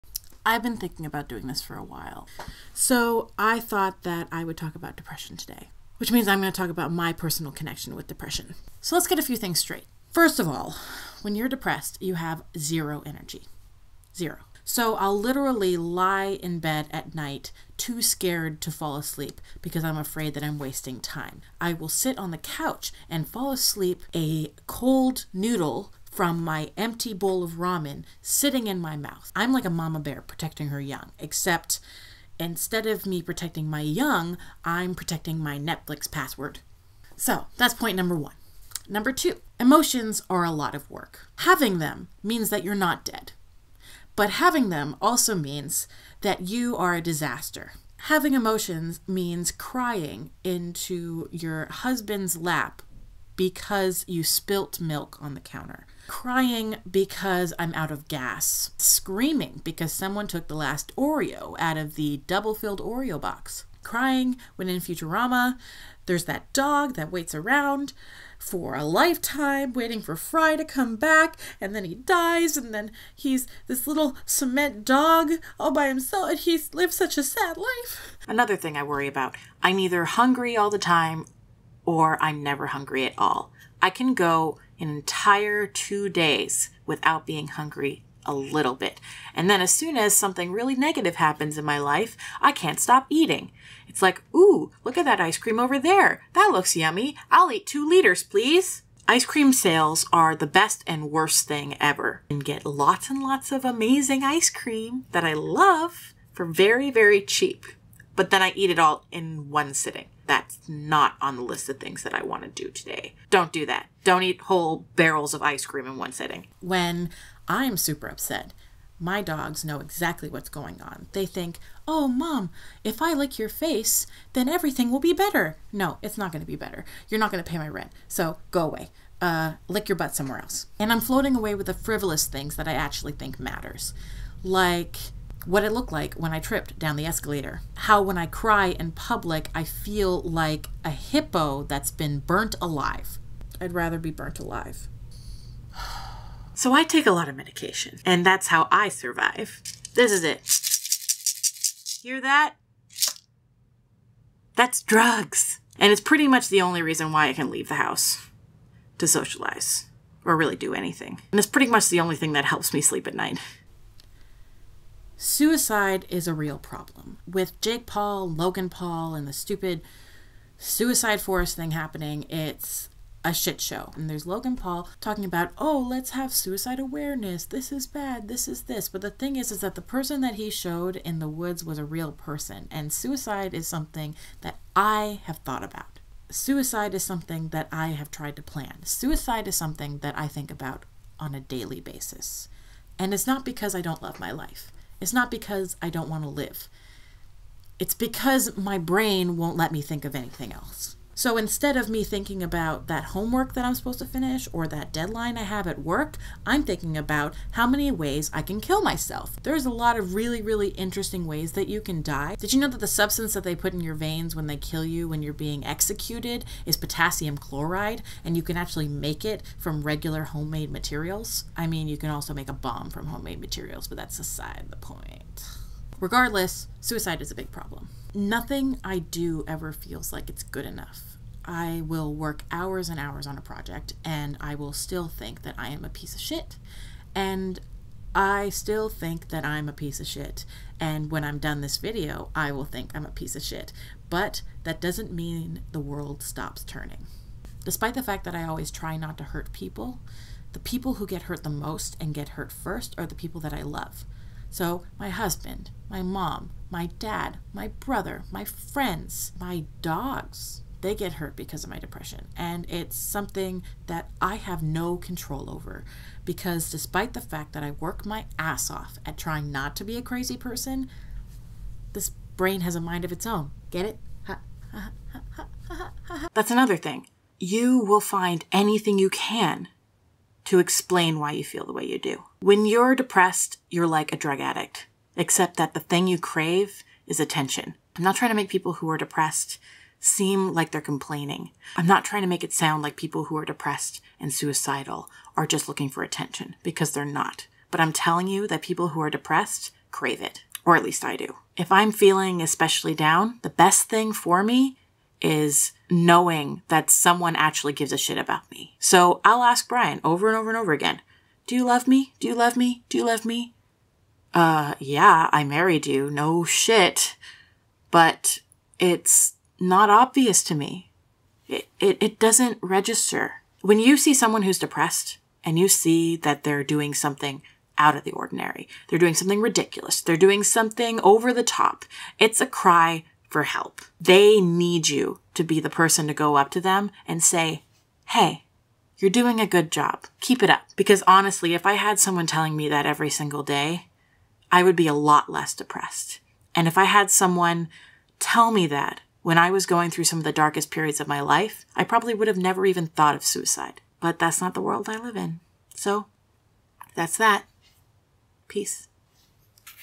What About Depression: My Side of the Story
This is a video that I have wanted to make for a while and I think that it's the perfect time to do it.

I have depression, anxiety, ptsd, ocd, and anything else you can come up with but I am doing my best to get by.

I have called a hotline and I can say without a doubt that it can and will save your life. Save the number in your phone.

To everyone out there, you deserve love. You deserve happiness. You deserve a future.